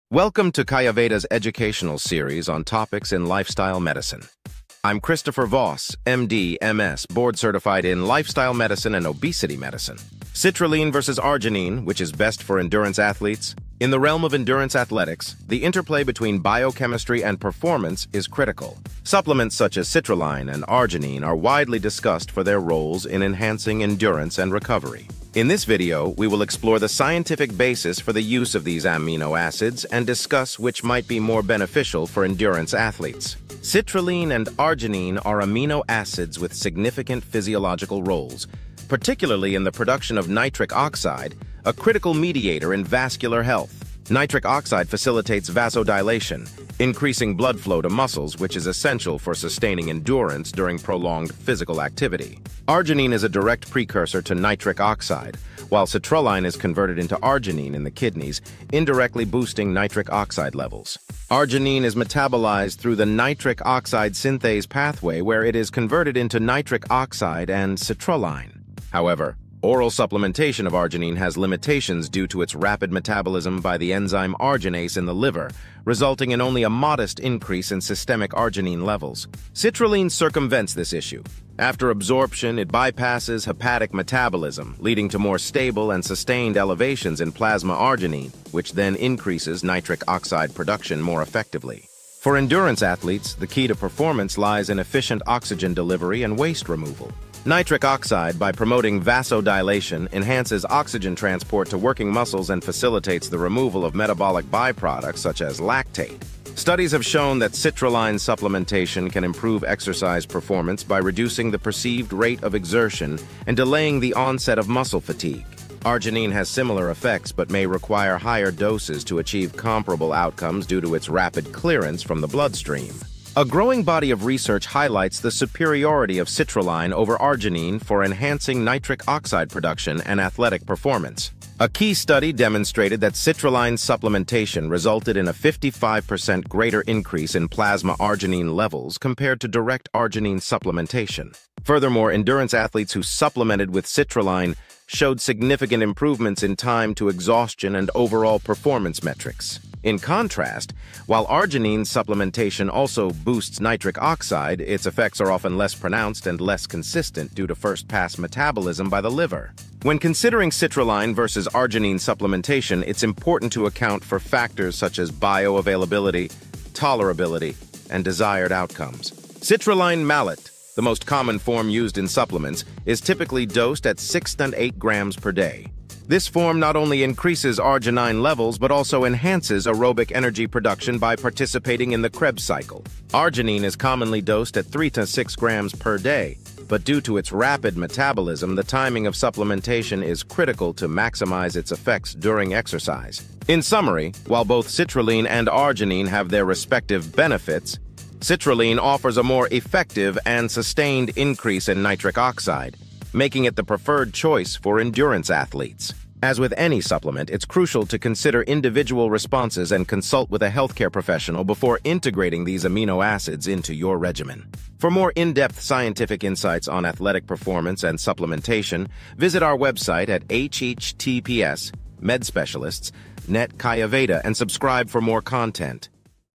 Go Beyond Your Endurance Boundaries - Citrulline vs. Arginine.
Citrulline vs. Arginine. Which is Best for Endurance Athletes?

Christopher Voss, MD, MS, FCCP
Board-certified in Lifestyle Medicine and Obesity Medicine

References

1. Schwedhelm E, Maas R, Freese R, et al. Pharmacokinetic and pharmacodynamic properties of oral l-citrulline and l-arginine: impact on nitric oxide metabolism. Br J Clin Pharmacol. 2008;65(1):51-59. doi:10.1111/j.1365-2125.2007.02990.x
2. Pérez-Guisado J, Jakeman PM. Citrulline malate enhances athletic anaerobic performance and relieves muscle soreness. J Strength Cond Res. 2010;24(5):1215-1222. doi:10.1519/JSC.0b013e3181cb28e0